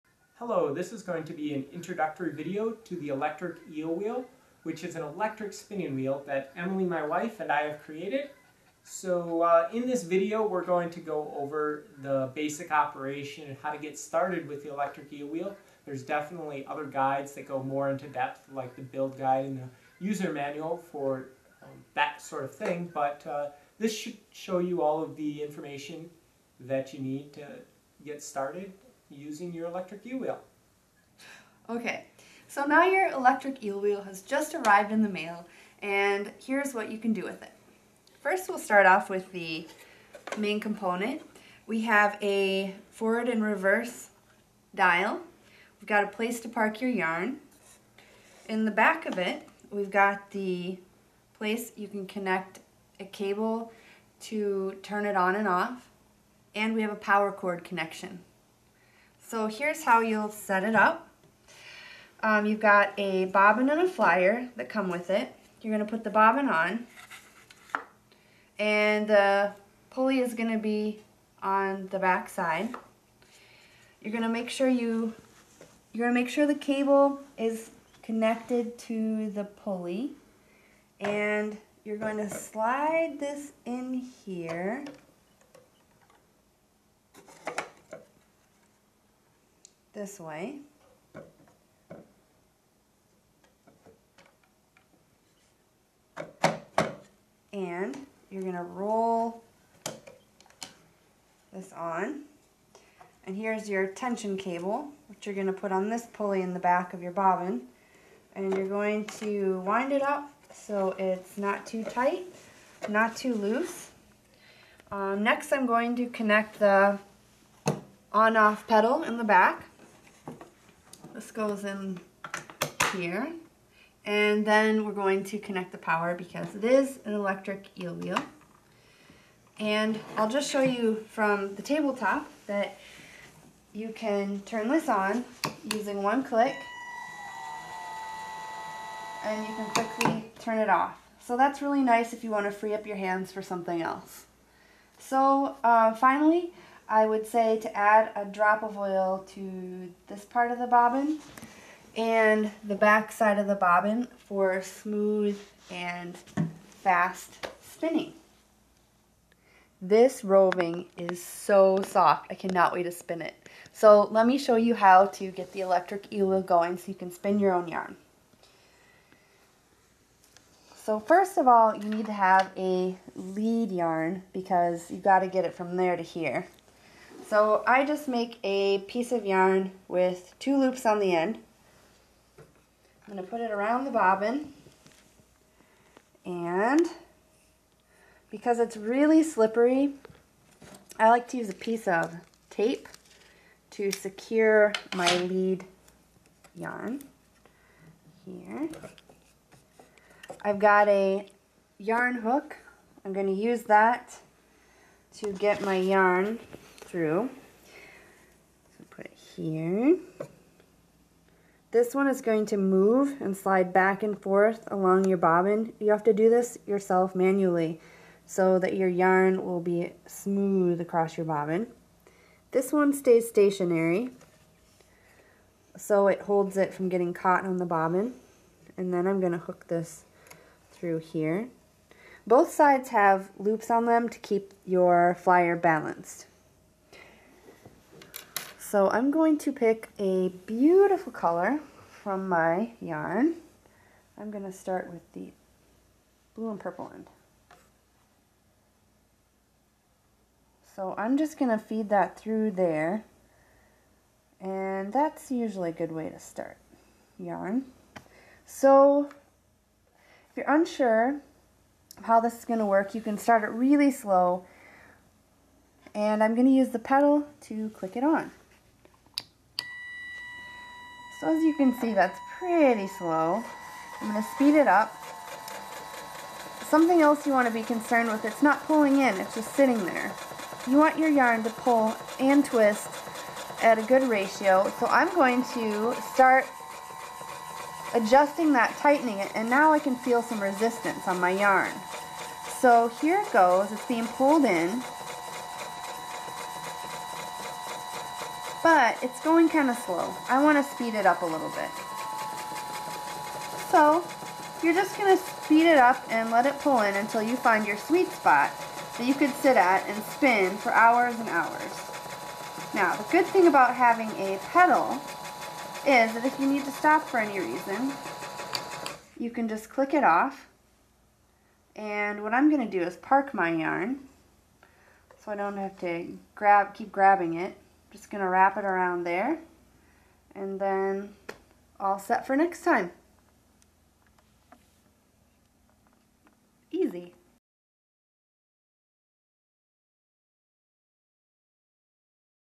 Electric Eel Wheel Overview
This video is an overview of using the Electric Eel Wheel version 3.  It is an electric spinning wheel that converts raw sheep, alpaca, or goat roving into yarn.